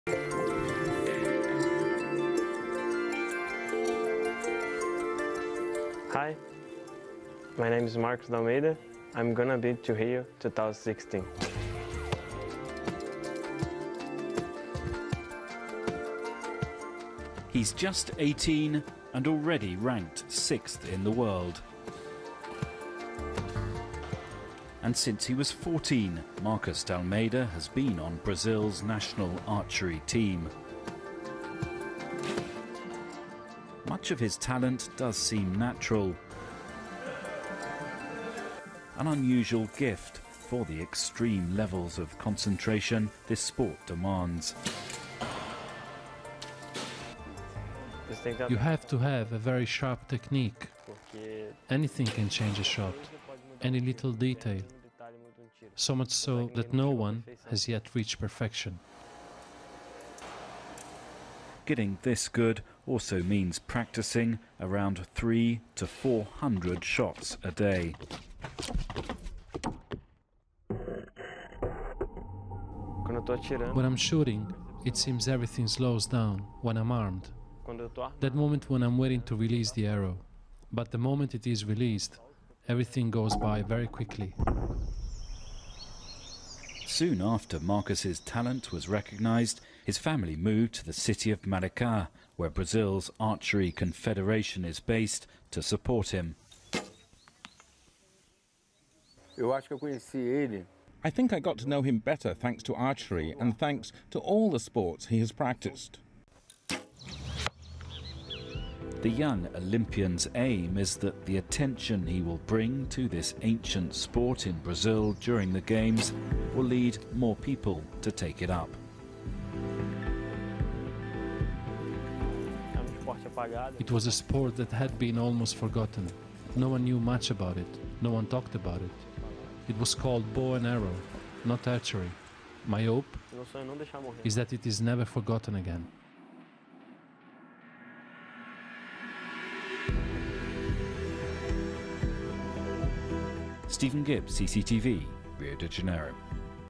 Archery hits a Brazilian bulls-eye at Rio Olympics
Skateboarding will be introduced at the next Summer Olympics in Tokyo. New disciplines are always vying for inclusion.
But one ancient pursuit is one of the oldest sports in the world.
CCTV America’s Stephen Gibbs reports.